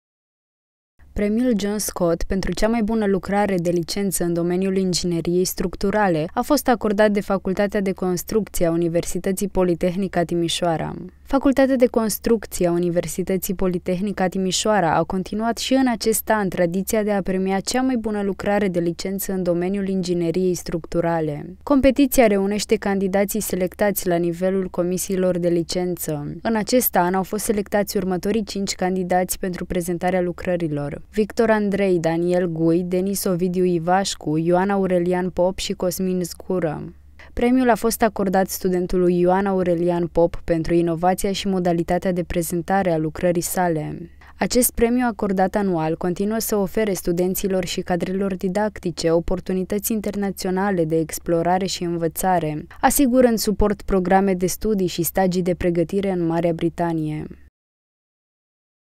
Premiul John Scott acordat la Timișoara
Premiul John Scott pentru cea mai bună lucrare de licență în domeniul ingineriei structurale, a fost acordat de Fcaultatea de Construcții a Universității Politehnica Timișoara. Facultatea de Construcții a Universității Politehnica Timișoara a continuat și în acest an tradiția de a premia cea mai bună lucrare de licență în domeniul ingineriei structurale. Competiția reunește candidații selectați la nivelul comisiilor de licență. În acest an, au fost selectați următorii cinci candidați pentru prezentarea lucrărilor,Victor Andrei, Daniel Gui, Denis Ovidiu Ivașcu, Ioan-Aurelian Pop și Cosmin Zgură.